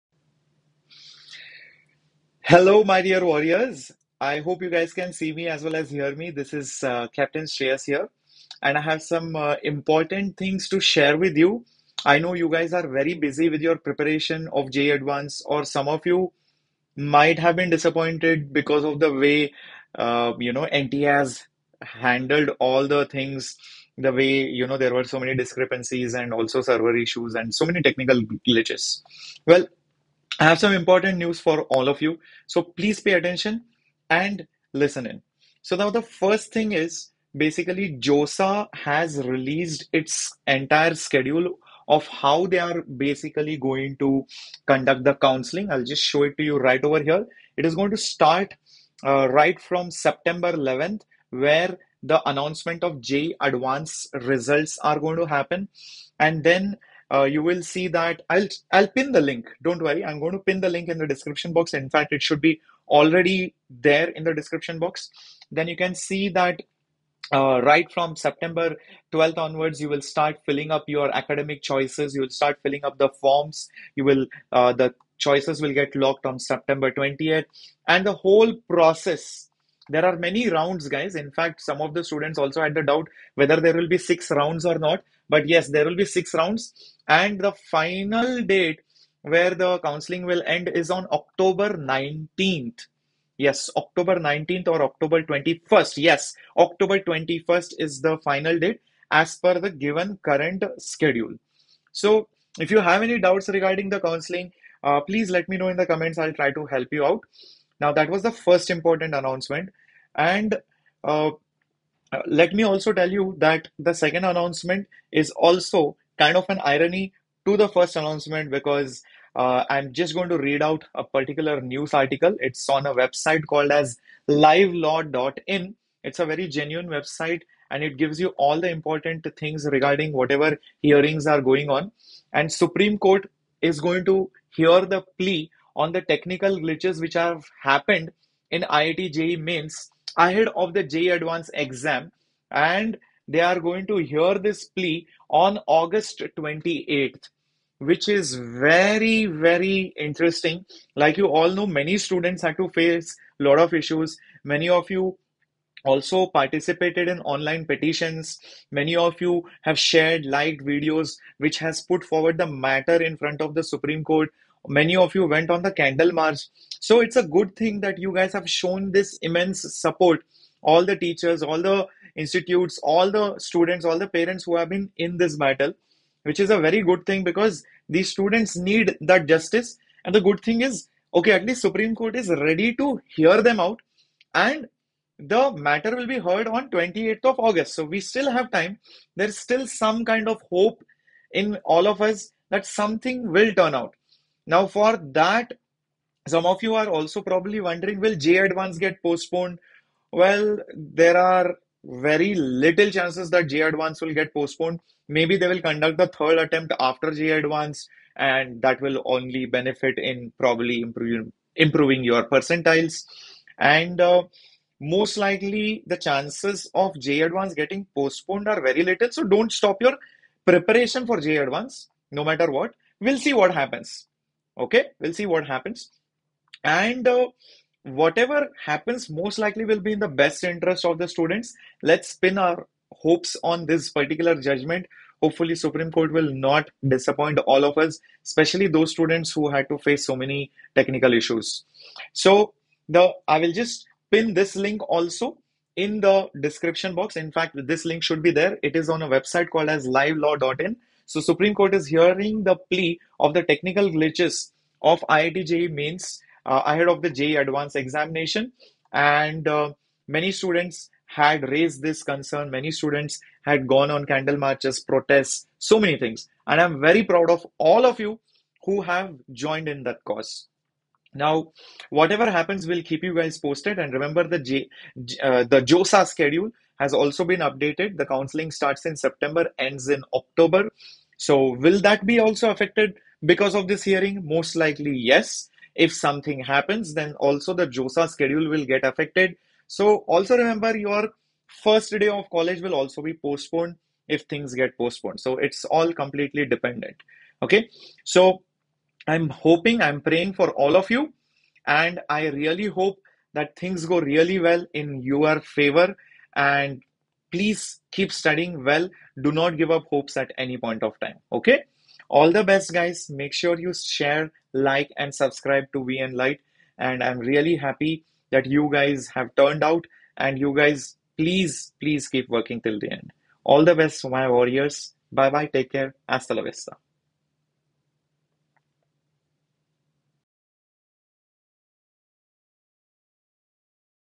JEE Mains 3rd attempt (Supreme court hearing) & JoSAA 2022 schedule | Shreyas sir
JEE Mains 3rd attempt & JoSAA 2022 schedule